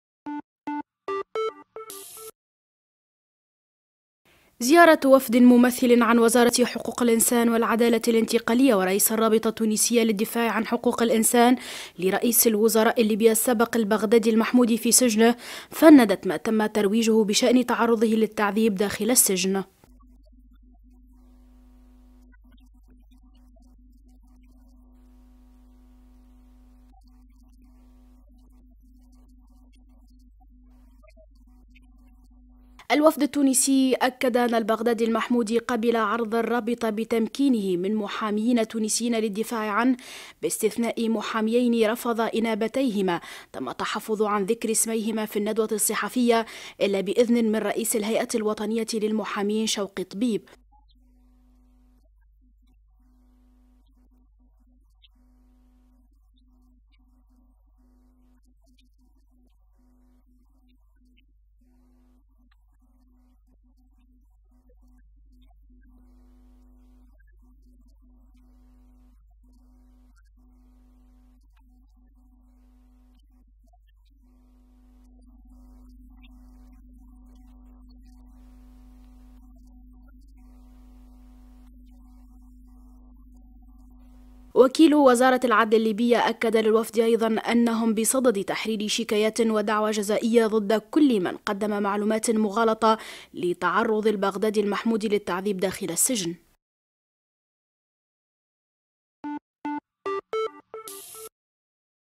البغدادي المحمودي ينفي تعرضه للتعذيب
نفى الوفد التونسي الذي زار رئيس الوزراء السابق البغدادي المحمودي في سجنه في ليبيا  و المتكون من ممثلين عن وزارة حقوق الانسان والعدالة الانتقالية ورئيس الرابطة التونسية للدفاع عن حقوق الانسان، تعرض البغدادي للتعذيب .كما أكد الوفد ان البغدادي قبل تكليف محاميين تونسيين للدفاع عنه باستثناء محاميين رفض الوفد الكشف عن اسميْهما خلال الندوة الصحفية.

تقرير:فتحية حرابي
تصوير:أحمد الصيد
مونتاج:ألفة نجاري